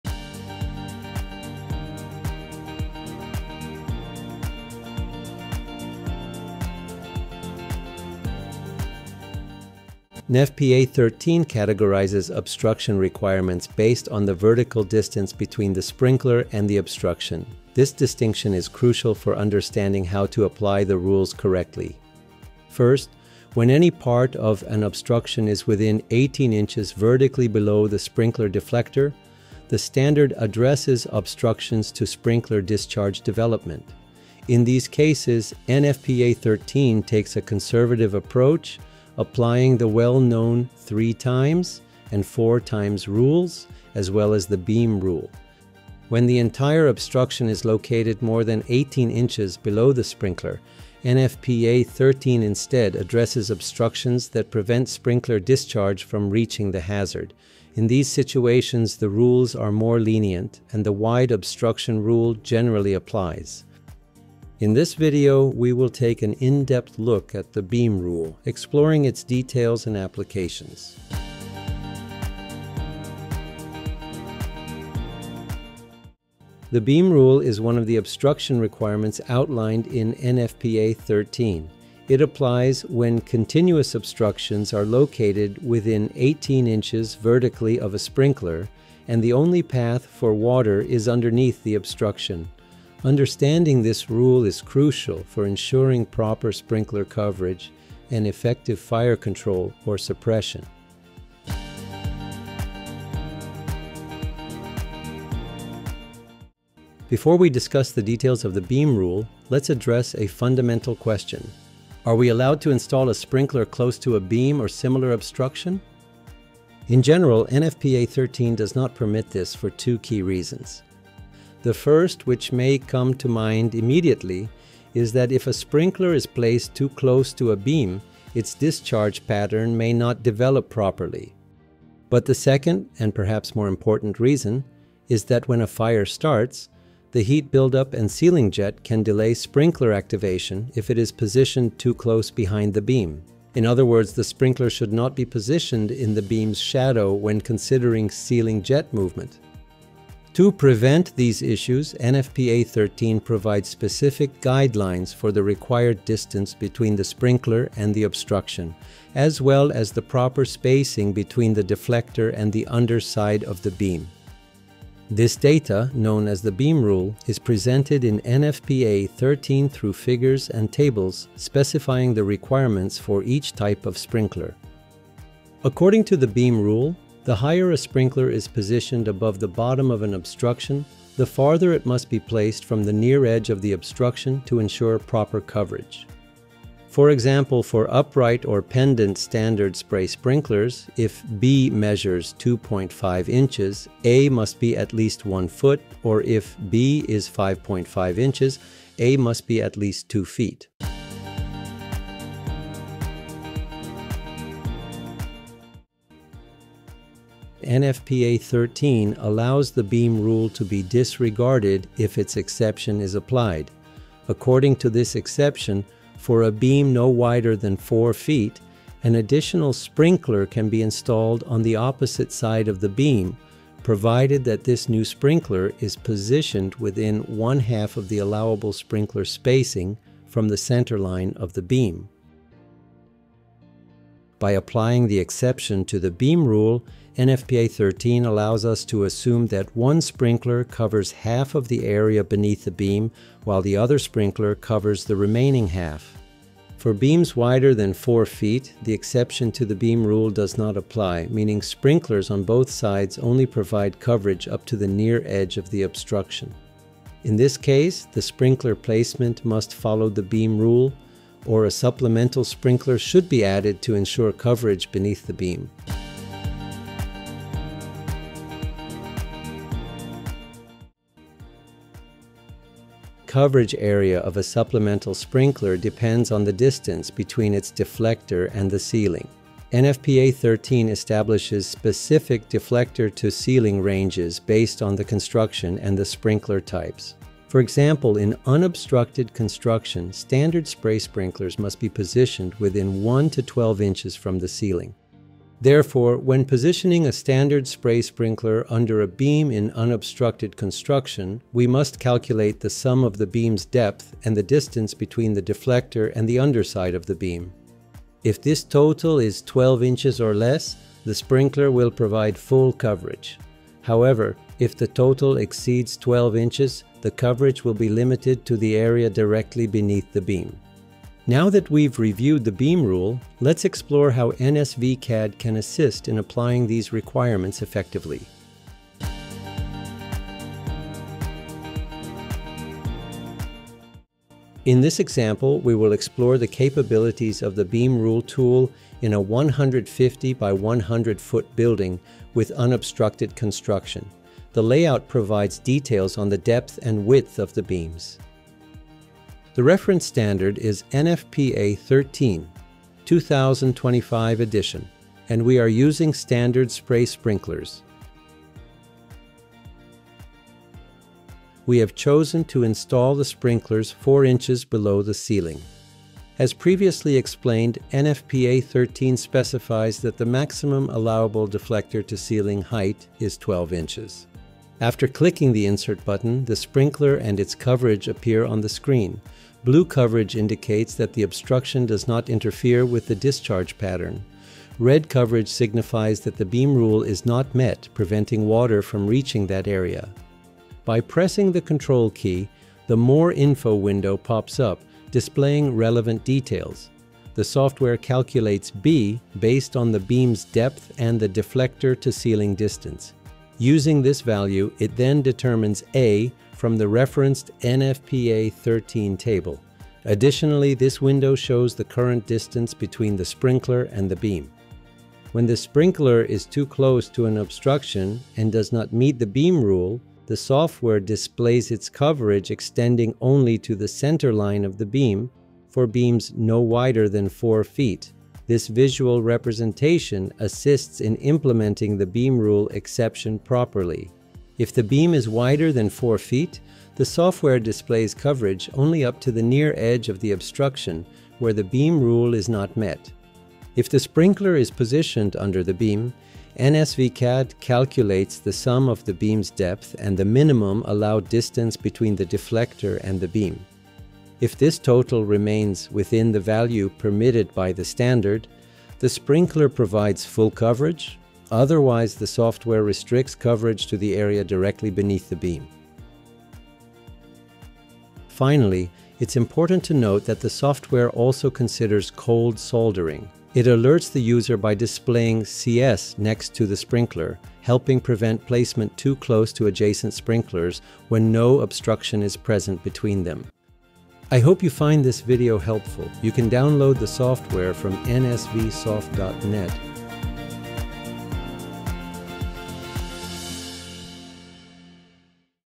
📌Beam Rule Explained: Sprinkler Obstruction (NFPA 13/13R/13D)
Proper sprinkler placement is crucial in fire protection systems, and NFPA 13, 13R, and 13D establish clear obstruction requirements to ensure optimal coverage.
In this video, we take an in-depth look at the Beam Rule, a key component of obstruction requirements. From general principles to exceptions, we break down essential considerations that impact sprinkler effectiveness.